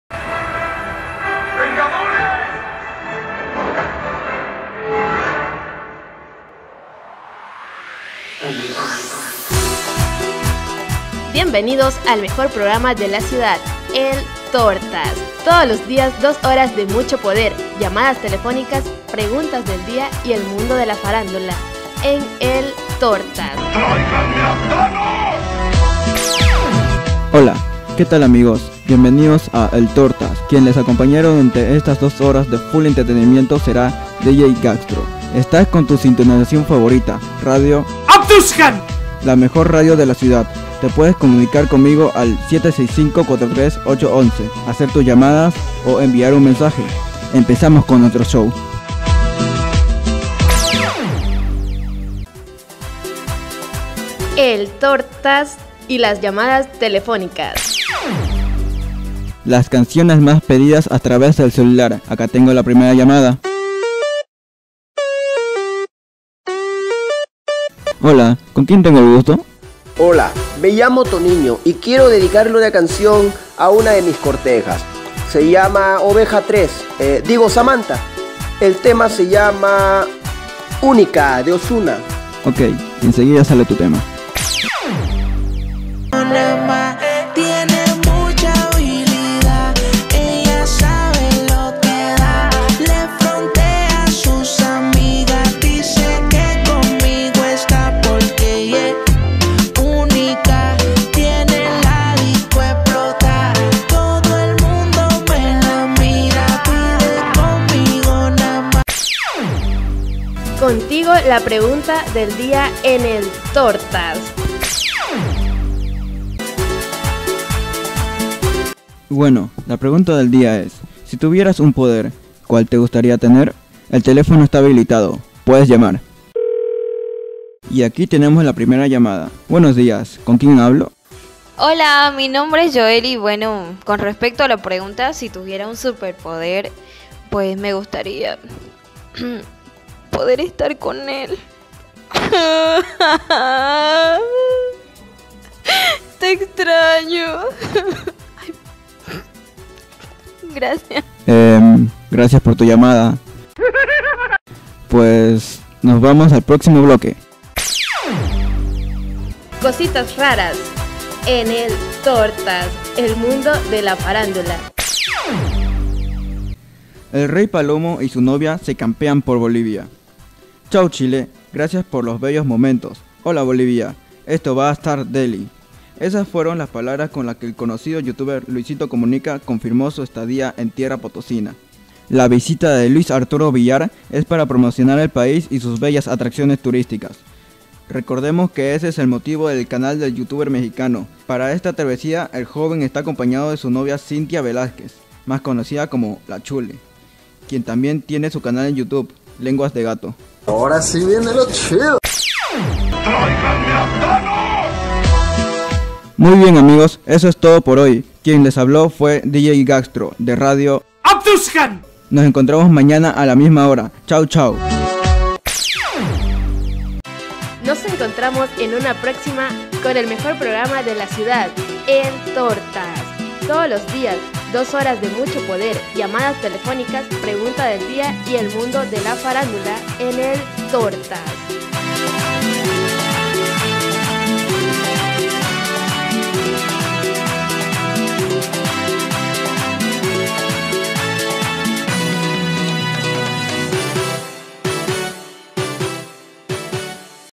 ¿Qué sùper poder quisieras tener? // EL THOR-TAS - Cap. 1
¡HEY!

Gracias por abrir xd
Este es un programa de radio que hice para el instituto. Le añadí vídeos varios para que sea un poco entretenido verlo y no sea solo el audio...
Si te gustò el vídeo y quieres que haga màs de este tipo, por favor, házmelo saber en los comentarios o simplemente deja tu like.